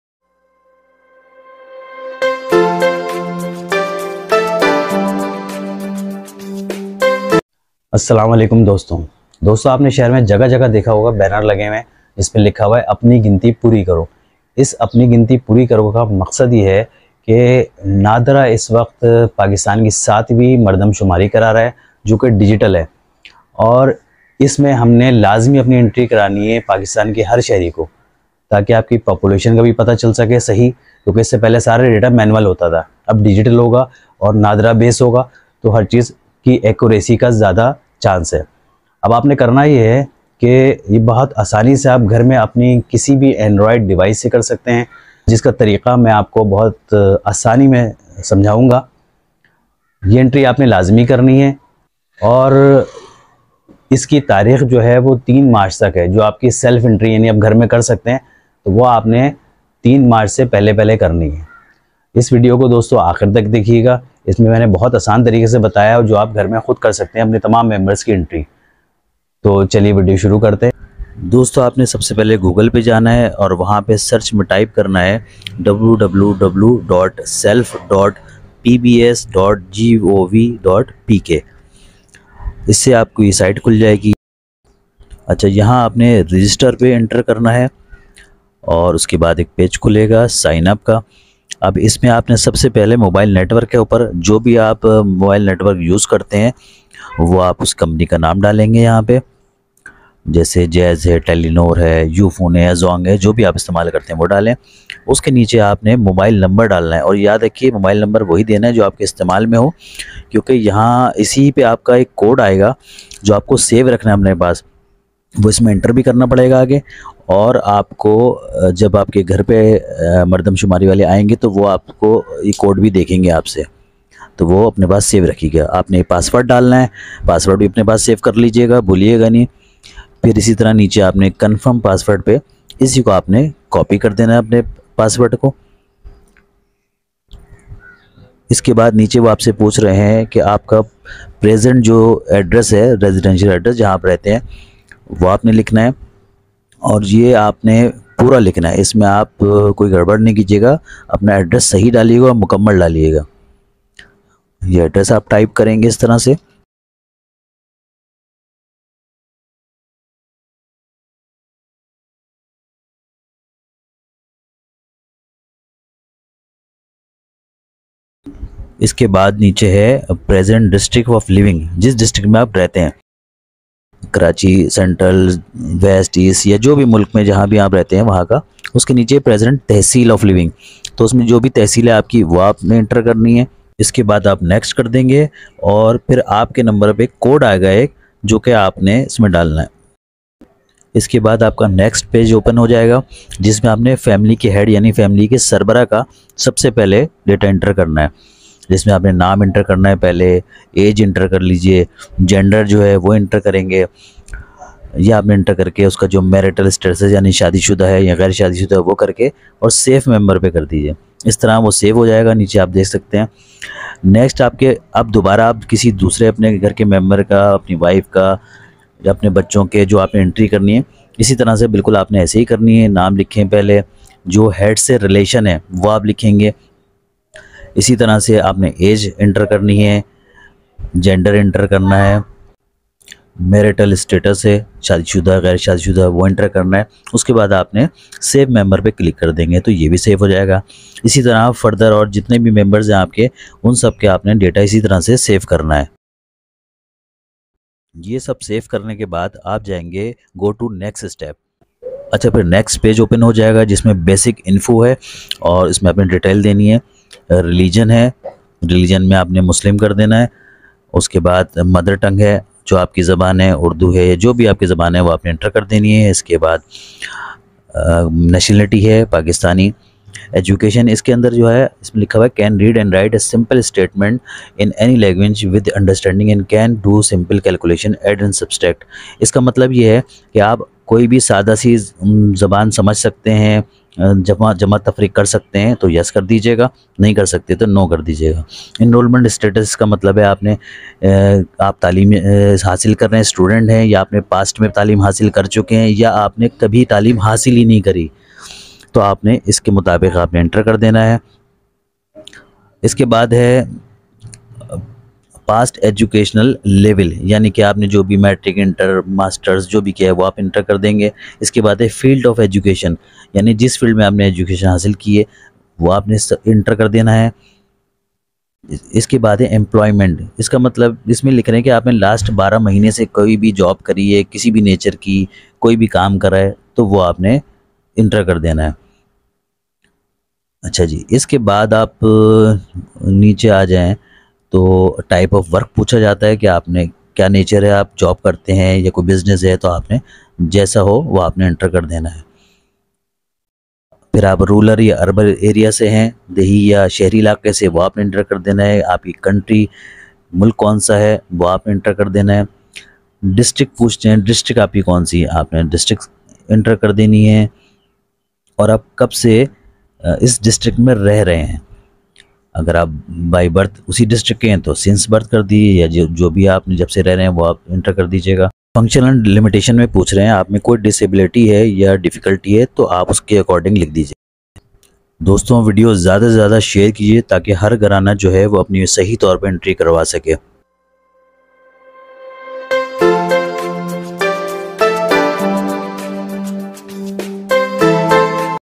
Apni ginti pori karo, 7th census of Pakistan, first time digital census in Pakistan مردم شماری
For the first time in Pakistan, NADRA proposed a comprehensive “IT Solution” to carry out 7th Population and Household Census of Pakistan, “THE DIGITAL CENSUS”. Span of this activity is covering the whole country, in 628 Tehsils comprising approx. 185,000 Census blocks. The activity shall be performed using android based smart devices, equipped with android based house listing and enumeration application synchronized with GPS & GIS.